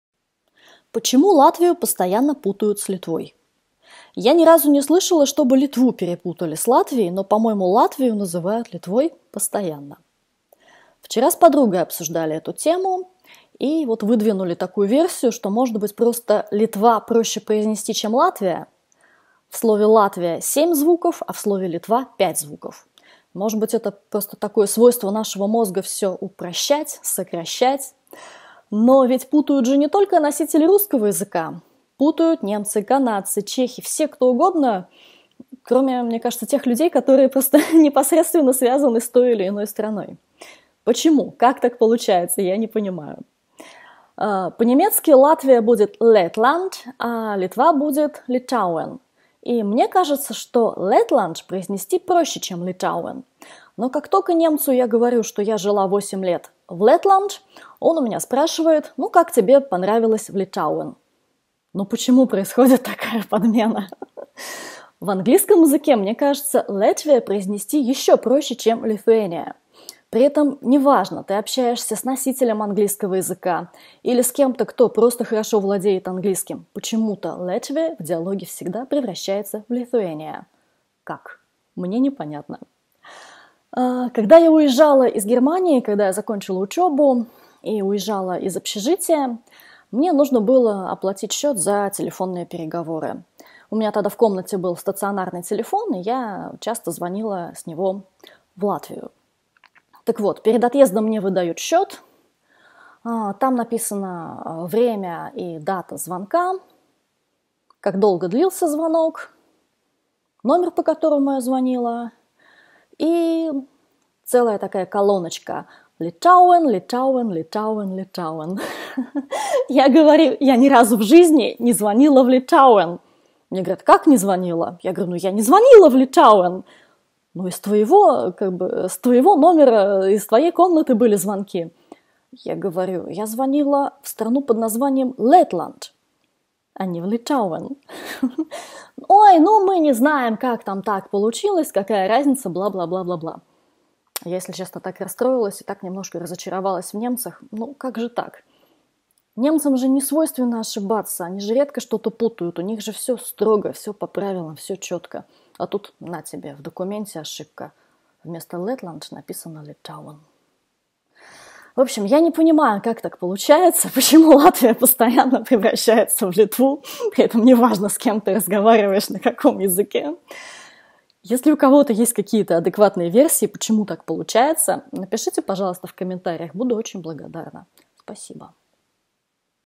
Latvia or Lithuania?! Почему Латвию путают с Литвой?
Почему Латвию постоянно путают с Литвой? У меня нет ответа на этот вопрос. Возможно, у Вас есть какая-то версия?